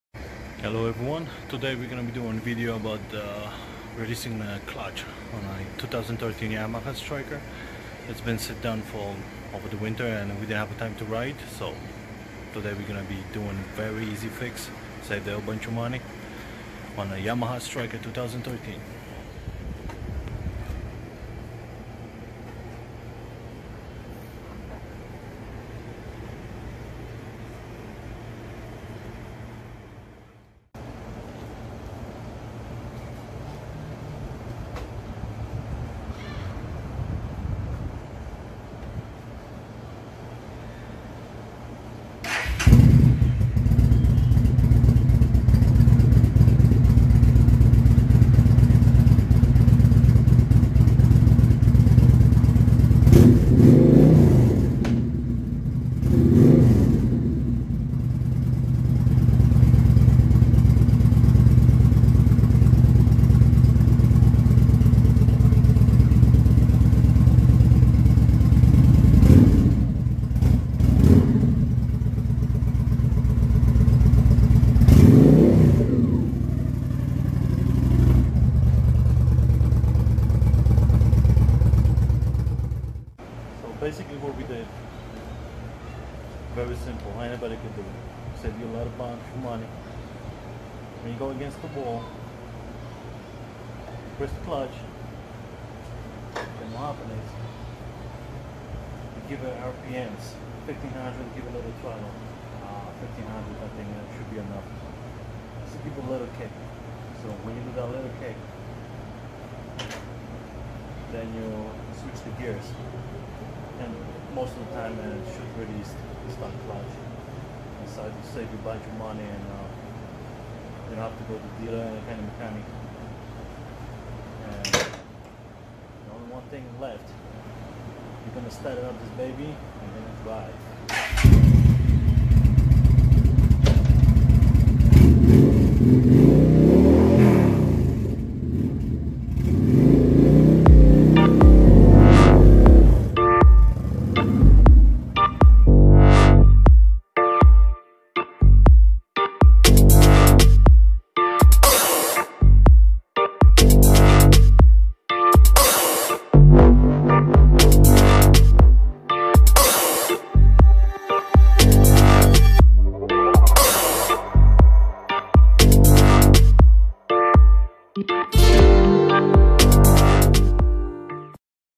How to free stuck clutch Yamaha Striker 2013
Very easy fix releasing stuck clutch  when you not riding for longer time